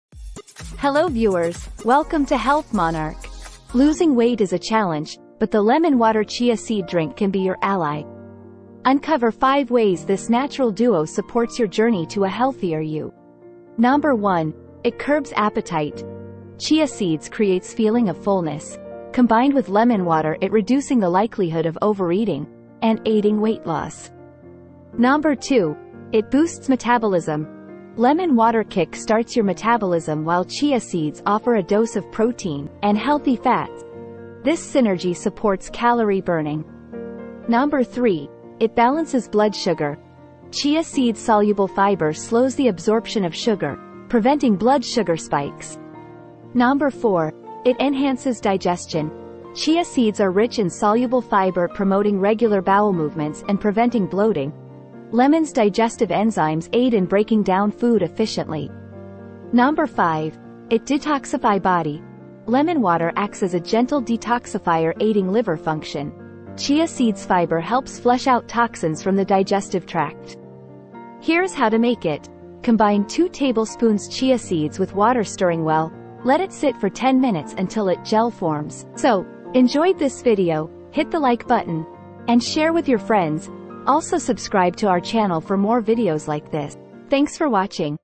One Glass In Morning Helps You Lose 5 Kgs Fast
🌟 In today's video, we're sharing an incredible morning drink that can help you shed 5 kgs fast! 🥤💪

If you're looking for a natural and effective way to boost your weight loss journey, this is the video for you. This simple yet powerful drink is easy to make and can be incorporated into your daily routine with minimal effort.

The secret ingredient that makes this drink so effective
Step-by-step instructions on how to prepare the drink
Tips on when and how to consume it for the best results
Additional lifestyle and dietary tips to enhance your weight loss
Remember, consistency is key! By incorporating this drink into your morning routine, you'll start seeing results in no time. Don't forget to pair this with a balanced diet and regular exercise for maximum effectiveness.